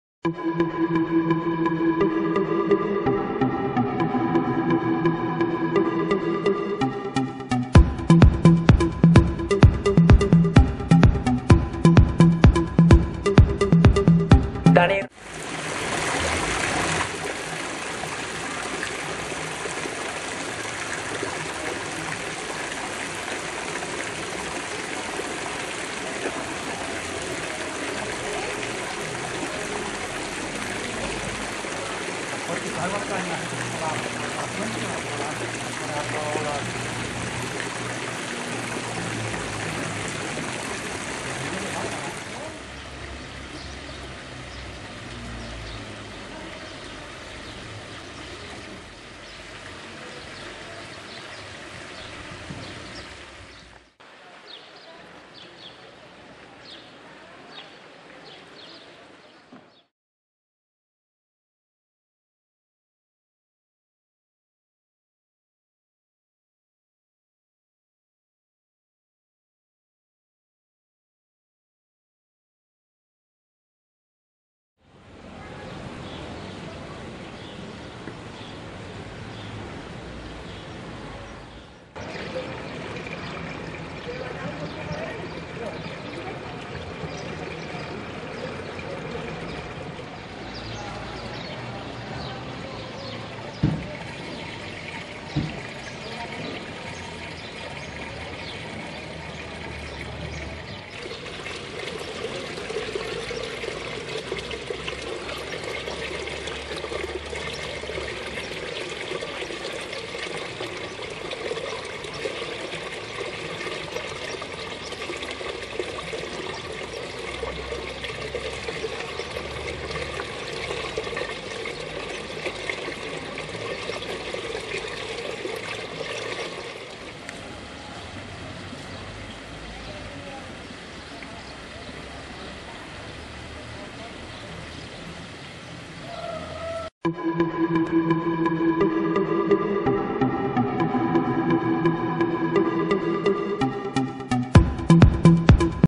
Montellano (Sevilla) Fuente del Lavadero Publico HD
Montellano (Sevilla) Fuente del Lavadero Publico
La Fuente y el Pilar como hace 40 años
Lastima que quitaran el  Lavadero Publico